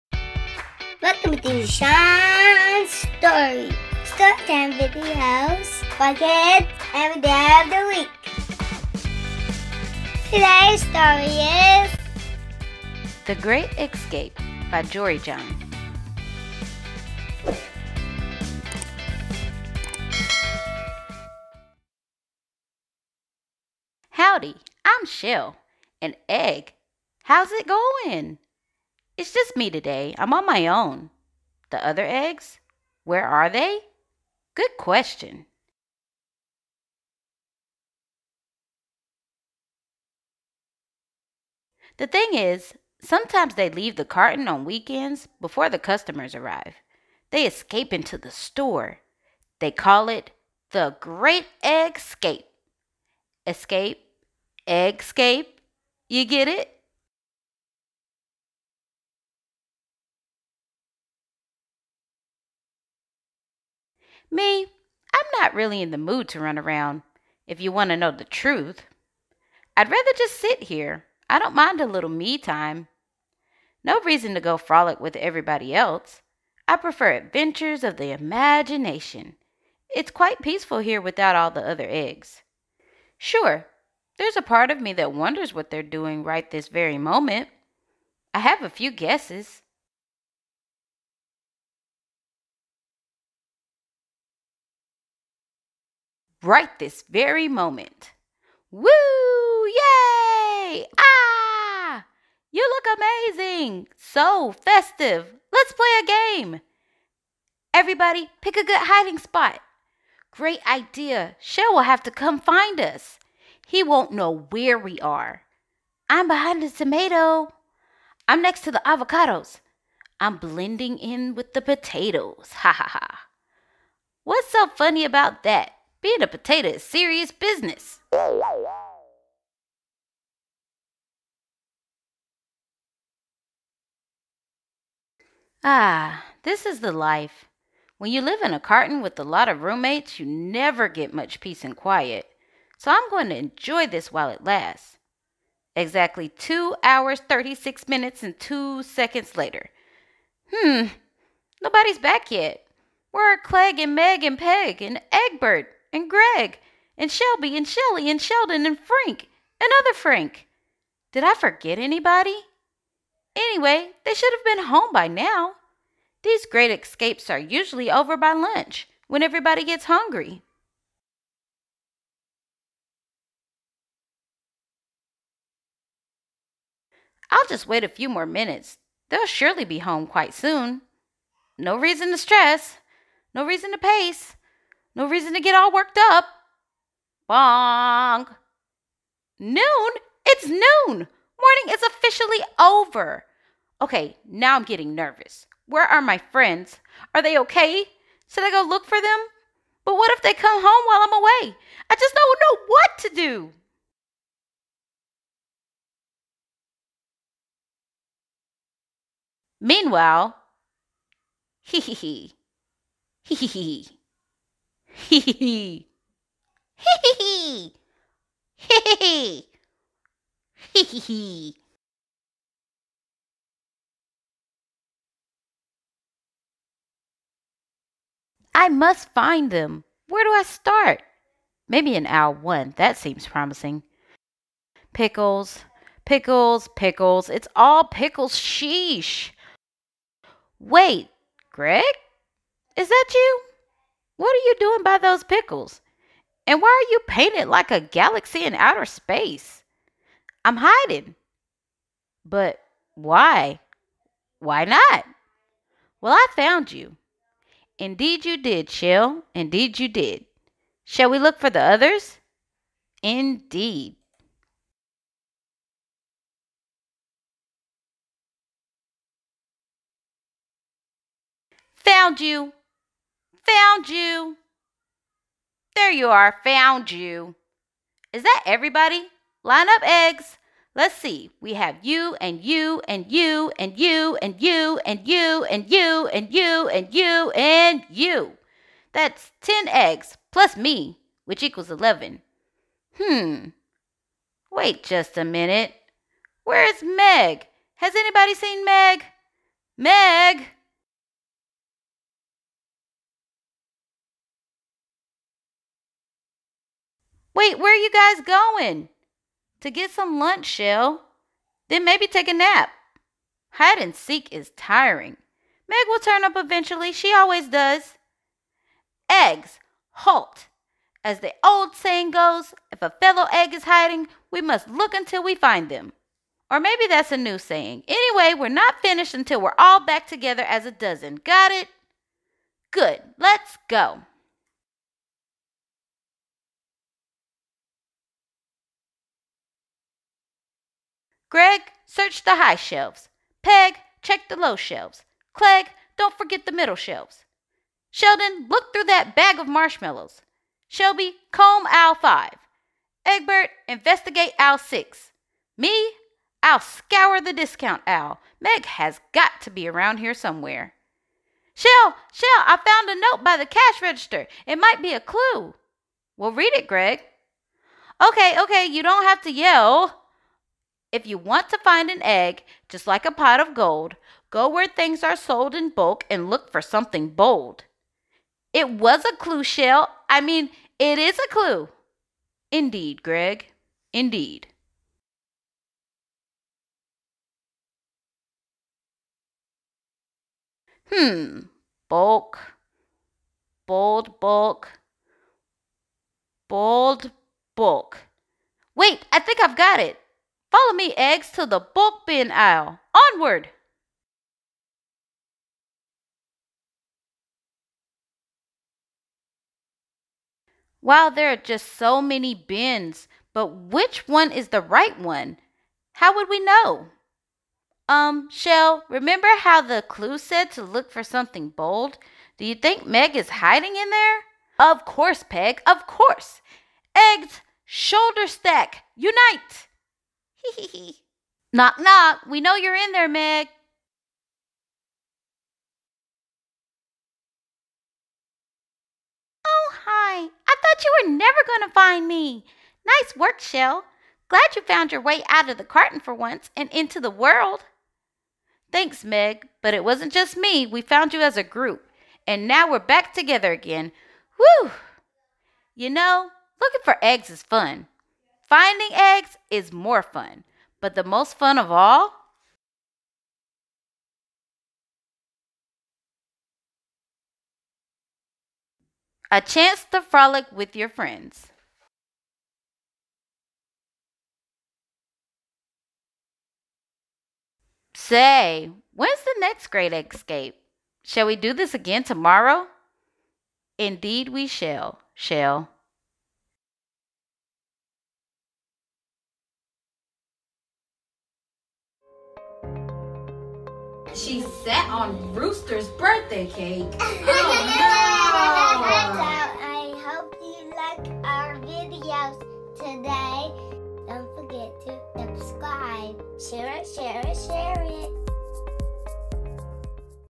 The Great Eggscape! | Read Aloud Story Time | Shon's Stories
The Great Eggscape! by Jory Jon read aloud.

Wanna connect with Shon?